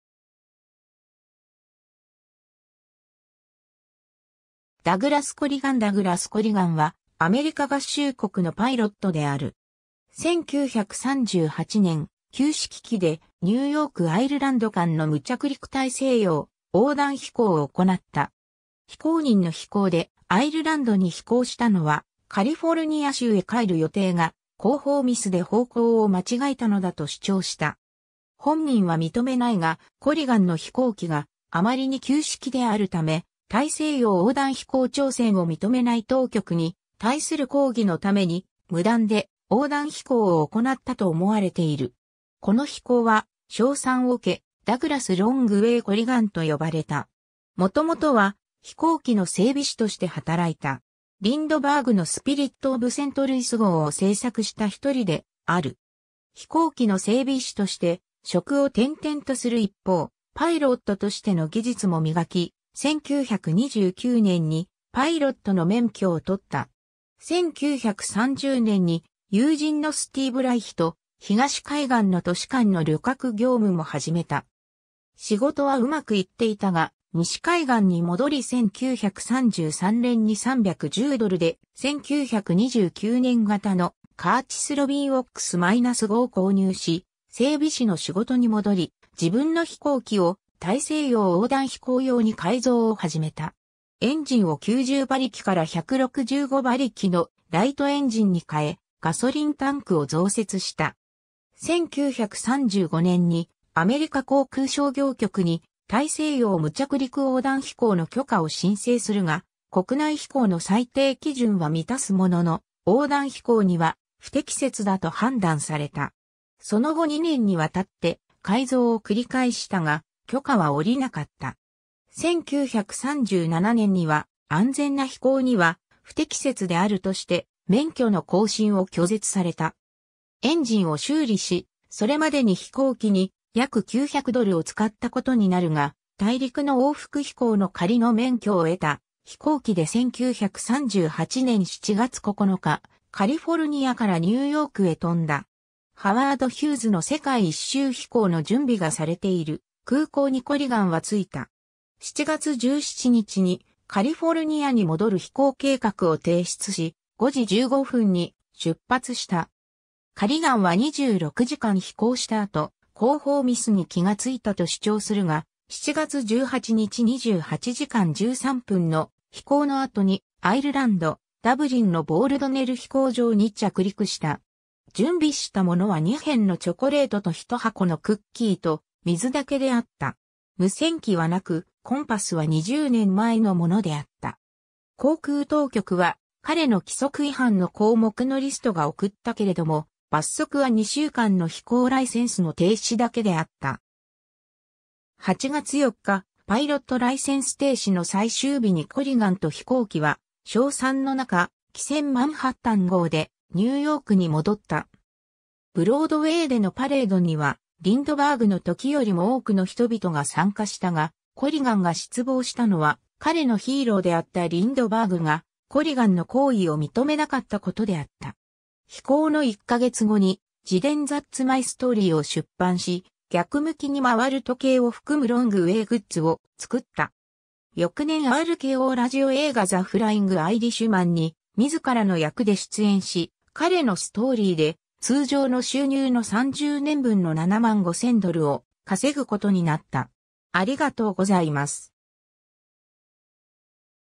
ダグラス・コリガン
ダグラス・コリガン ダグラス・コリガン（Douglas Corrigan、1907年1月22日 - 1995年12月9日）は、アメリカ合衆国のパイロットである。
1938年、旧式機でニューヨーク－アイルランド間の無着陸大西洋横断飛行を行った。
非公認の飛行で、アイルランドに飛行したのは、カリフォルニア州へ帰る予定が、航法ミスで方向を間違えたのだと主張した。
本人は認めないが、コリガンの飛行機があまりに旧式であるため、大西洋横断飛行挑戦を認めない当局に対する抗議のために、無断で横断飛行を行ったと思われている。
この飛行は賞賛をうけ、Douglas "Wrong Way" Corriganと呼ばれた。
 もともとは飛行機の整備士として働いた。
リンドバーグのスピリットオブセントルイス号を製作した1人である。
飛行機の整備士として職を転々とする一方、パイロットとしての技術も磨き、1929年にパイロットの免許をとった。
1930年に友人のSteve Reichと東海岸の都市間の旅客業務も始めた。
仕事はうまくいっていたが､西海岸に戻り1933年に310ドルで、1929年型のカーチスロビンOX-5を購入し、整備士の仕事に戻り、自分の飛行機を大西洋横断飛行用に改造を始めた。
エンジンを90馬力から165馬力のライトエンジンに換え、ガソリンタンクを増設した。
1935年にアメリカ航空商業局に大西洋無着陸横断飛行の許可を申請するが国内飛行の最低基準はみたすものの、横断飛行には不適切だと判断された。
その後2年に渡って改造を繰り返したが、許可はおりなかった。
1937年には、安全な飛行には不適切であるとして免許の更新を拒絶された。
エンジンを修理し、それまでに飛行機に約900ドルをつかったことになるが、大陸の往復飛行の仮の免許を得た飛行機で1938年7月9日カリフォルニアからニューヨークへ飛んだ。
ハワード・ヒューズの世界一周飛行の準備がされている空港にコリガンは着いた。
7月17日にカリフォルニアに戻る飛行計画を提出し、5時15分に出発した。
カリガンは26時間飛行した後、航法ミスに気が付いたと主張するが、7月18日28時間13分の飛行の後にアイルランド、ダブリンのボールドネル飛行場に着陸した。
準備したものは2片のチョコレートと1箱のクッキーと水だけであった。
無線機はなく、コンパスは20年前のものであった。
航空当局は彼の規則違反の項目のリストが送ったけれども、罰則は2週間の飛行ライセンスの停止だけであった。
 8月4日、パイロットライセンス停止の最終日にコリガンと飛行機は賞賛の中、気船マンハッタン号でニューヨークに戻った。
ブロードウェイでのパレードにはリンドバーグの時よりも多くの人々が参加したが、コリガンが失望したのは彼のヒーローであったリンドバーグがコリガンの行為を認めなかったことであった。
 飛行の1ヶ月後に自伝That's My Storyを出版し、逆向きに回る時計を含む'wrong-way'グッズを造った。
翌年RKO Radio映画The Flying Irishmanに自らの役で出演し、彼のストーリーで通常の収入の30年分の75,000ドルを稼ぐことになった。